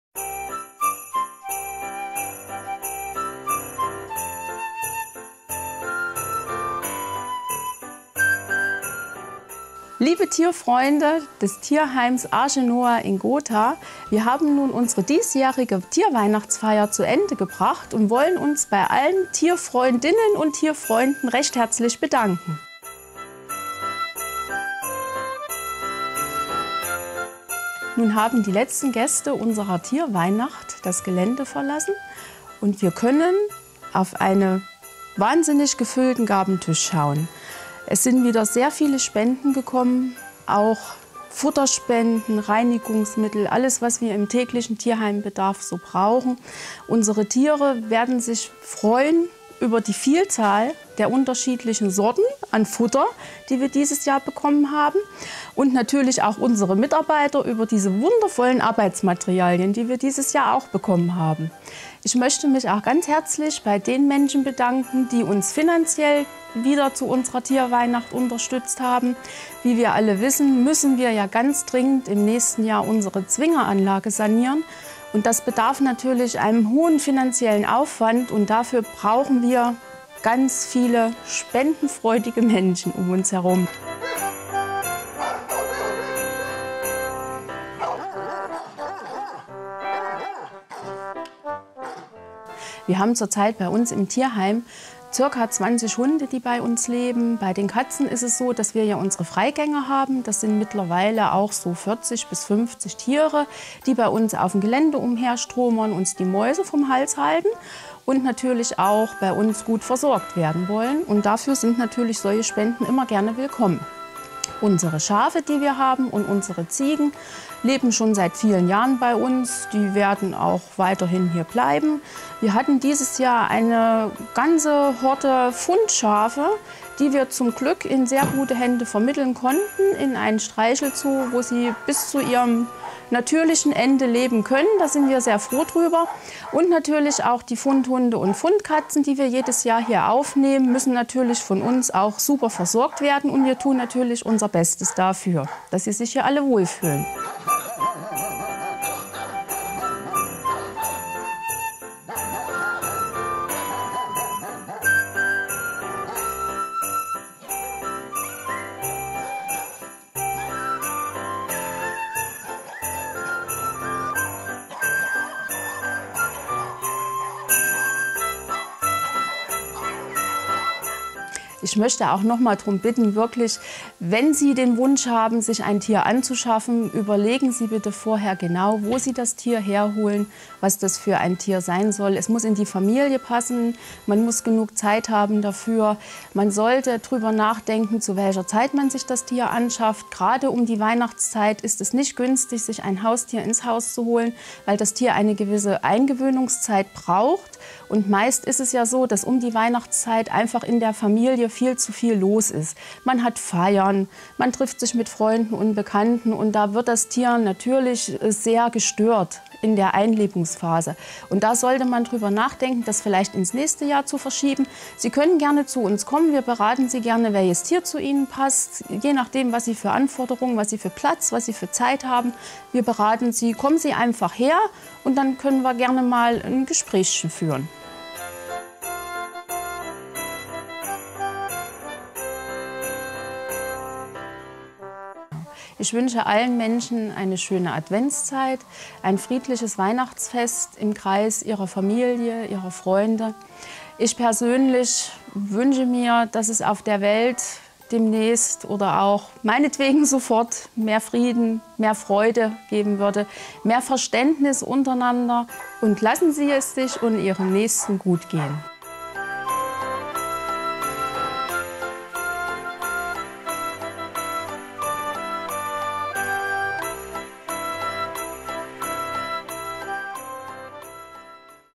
Tierweihnacht 2017
Am Samstag, dem 2. Dezember gab es im Tierheim "Arche Noah" Gotha die ebenso traditionelle wie beliebte Tierweihnacht. Wie jedes Jahr kamen viele Besucher und hinterließen Futter-, Sach- und Geldspenden.Tierheim-Leiterin Kathrin Matthieß zieht am Ende des Tages eine Bilanz. Mehr Informationen im Tierheim Gotha unter Tel. 03621/ 755 425.